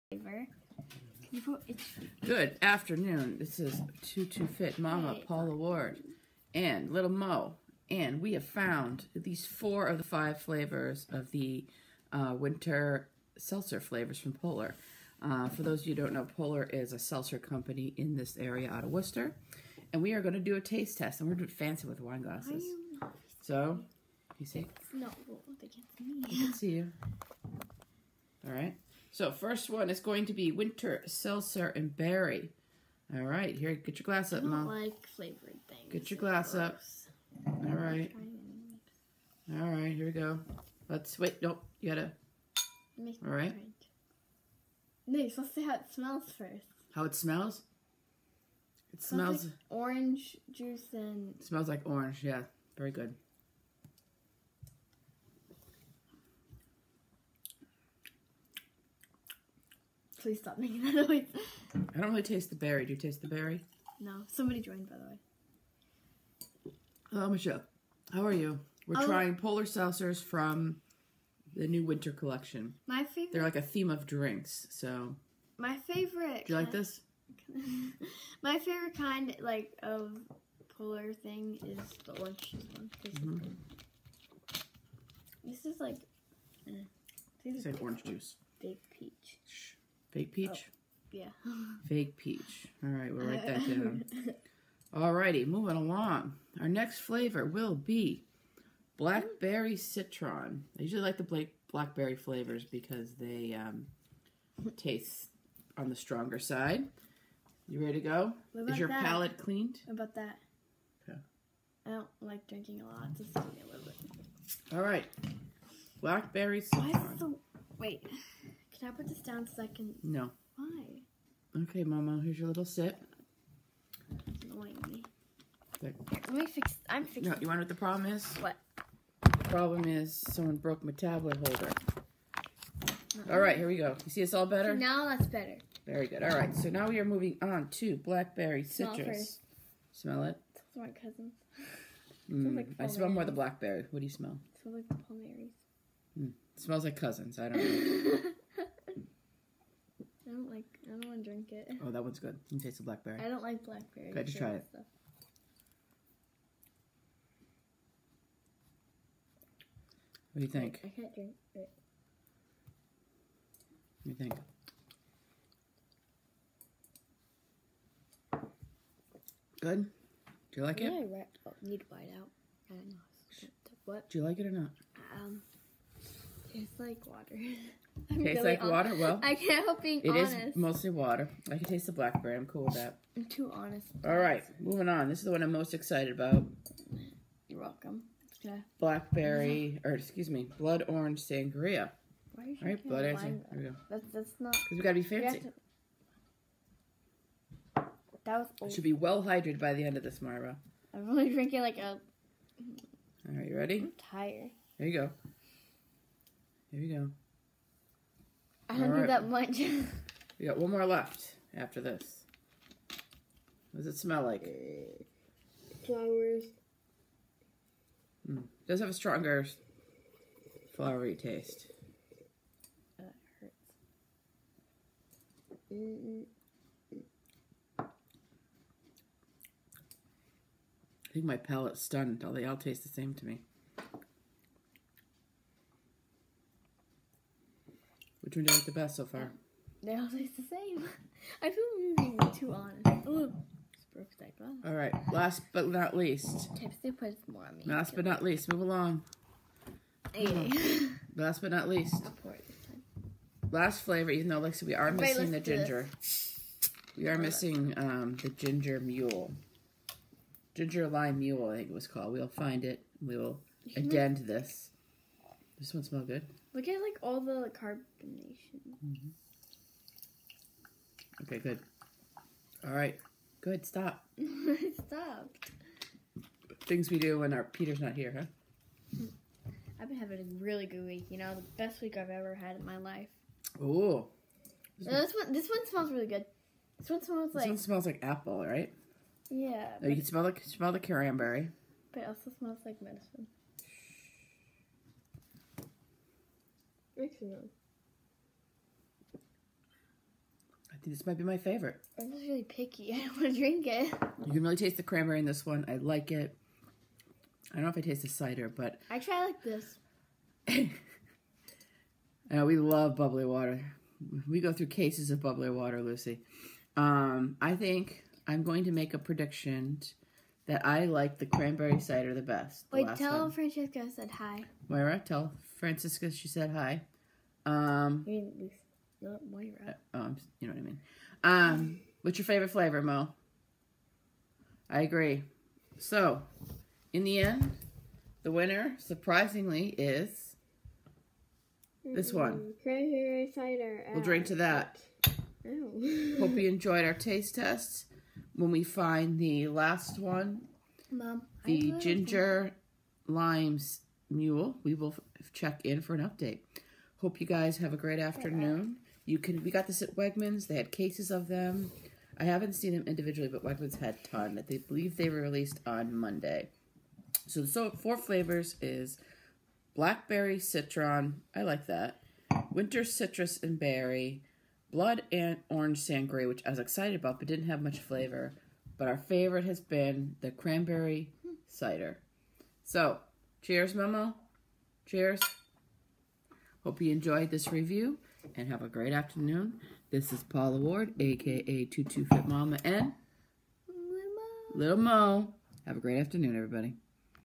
2017 winter polar seltzer taste testing
Hot Mess Chef and Lil Mo review the new winter flavors for polar!! Listen to our live review.
*For complete blog check out *Please check out my Facebook like